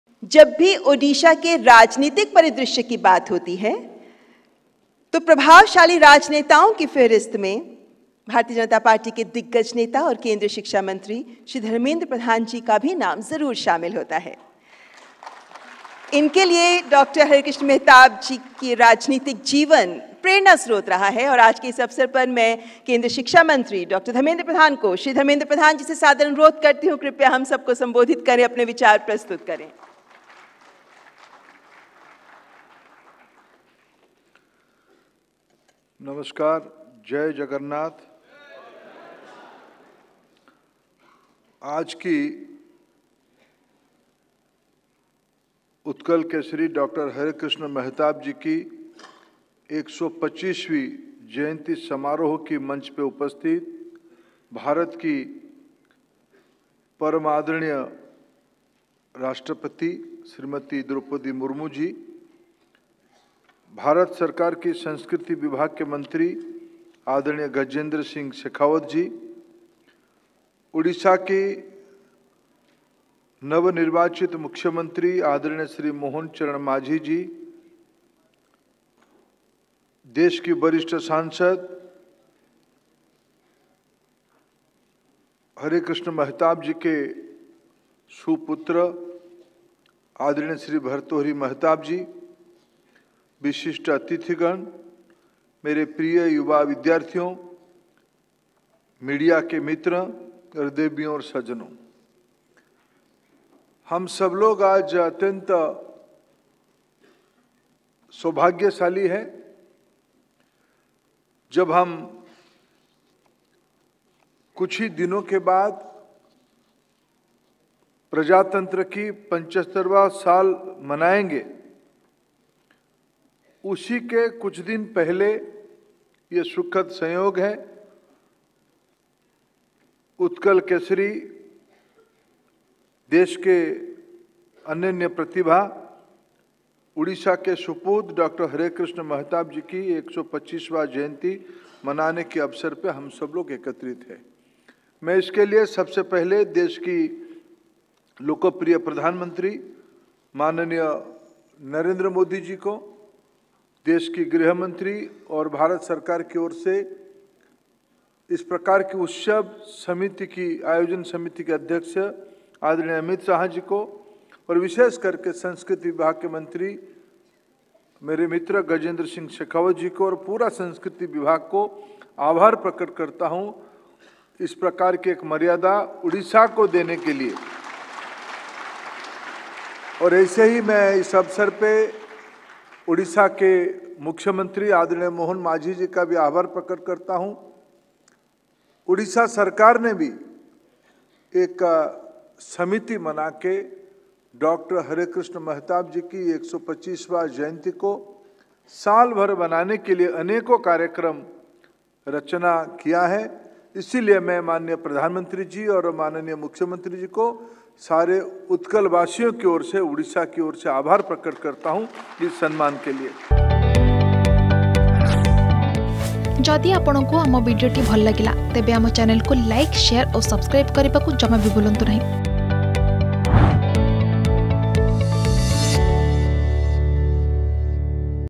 ଉତ୍କଳକେଶରୀଙ୍କ ୧୨୫ତମ ଜୟନ୍ତୀରେ ଯୋଗଦେଲେ କେନ୍ଦ୍ର ଶିକ୍ଷାମନ୍ତ୍ରୀ ଧର୍ମେନ୍ଦ୍ର ପ୍ରଧାନ || New Delhi
ଉତ୍କଳକେଶରୀଙ୍କ ୧୨୫ତମ ଜୟନ୍ତୀରେ ଯୋଗଦେଲେ କେନ୍ଦ୍ର ଶିକ୍ଷାମନ୍ତ୍ରୀ ଧର୍ମେନ୍ଦ୍ର ପ୍ରଧାନ ।